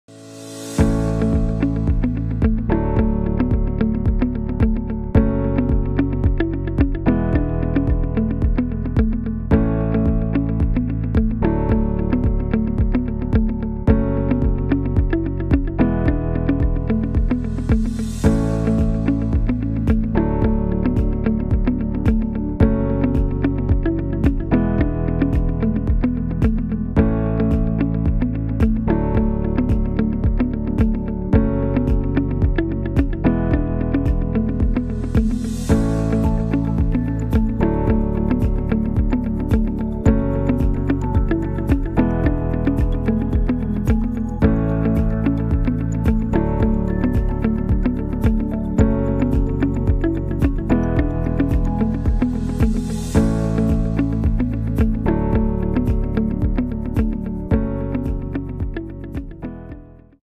Osiyo Xalqaro Universiteti o'qituvchisi Jo'raqulov Sanjar darslaridan lavhalar
@Osiyo.Xalqaro.Universiteti. Umumtexnika kafedrasi o'qituvchisi Jo'raqulov Sanjar Fizika fanidan lavhalar